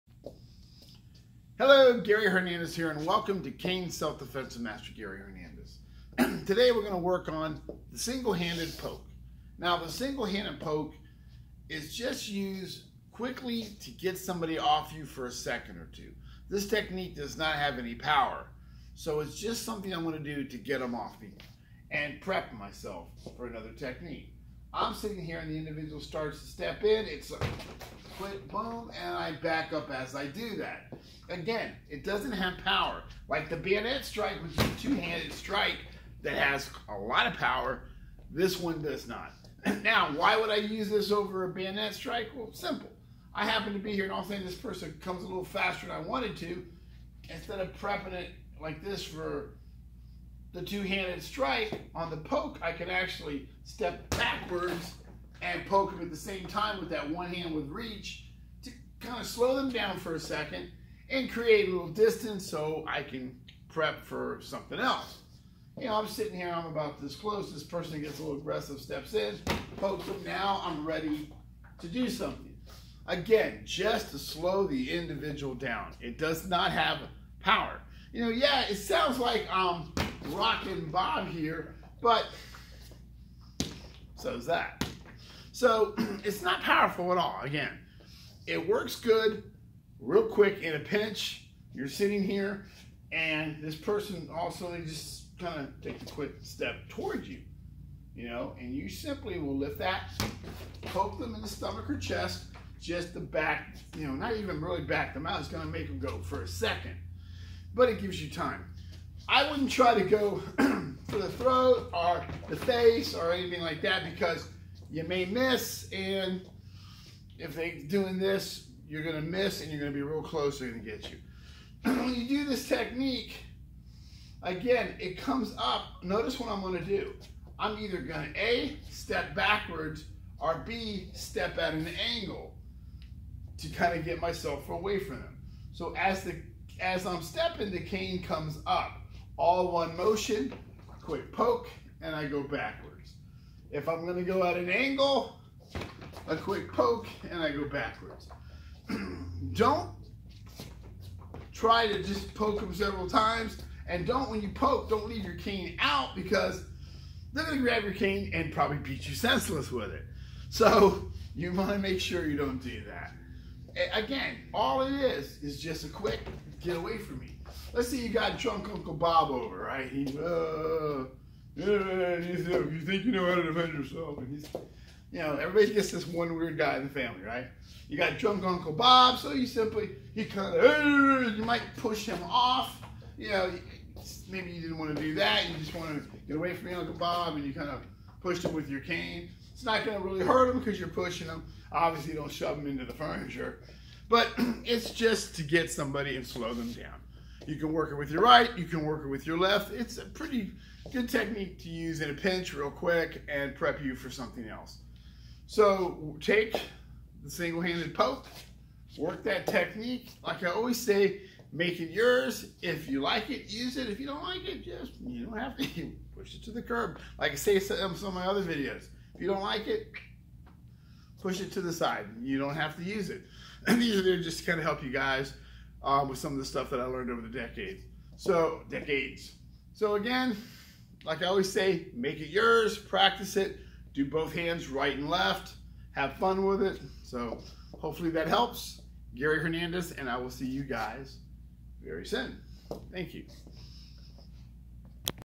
Cane self-defense with the single handed poke
Master Gary Hernandez with cane self-defense on the single handed poke.